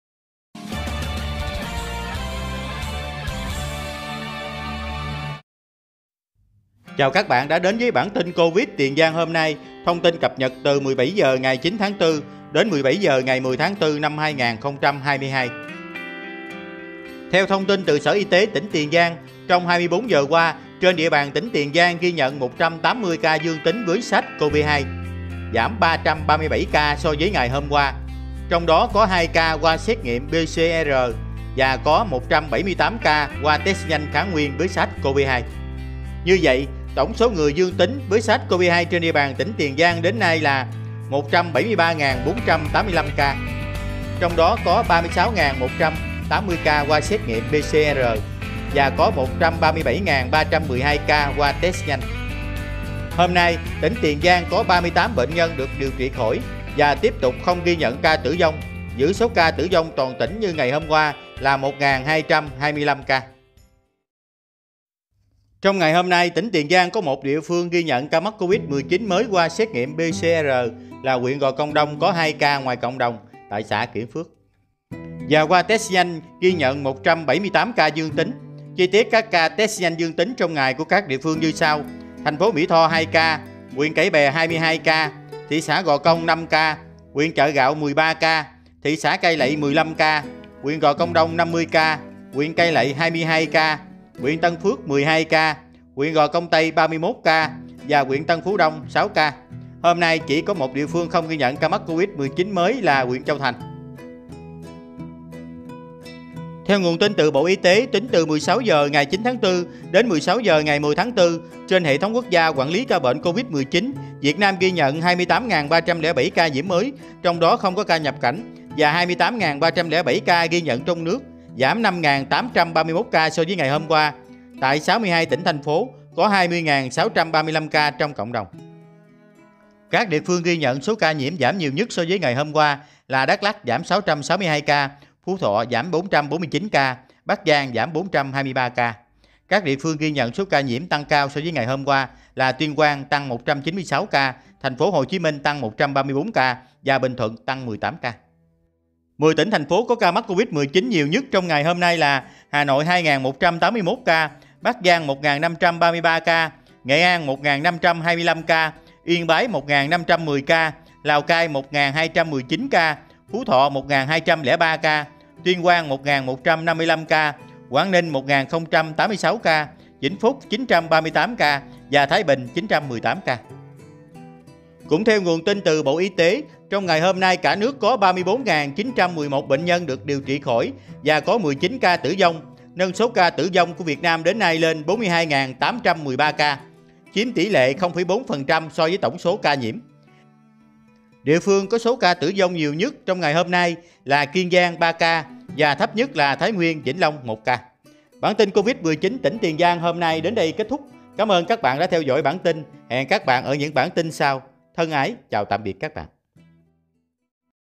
TIN TỨC COVID TIỀN GIANG 10/4:2 FO PCR,178 CA TEST NHANH DƯƠNG TÍNH
theo nguồn tin tuyengiaotiengiang.vn
bộ y Tế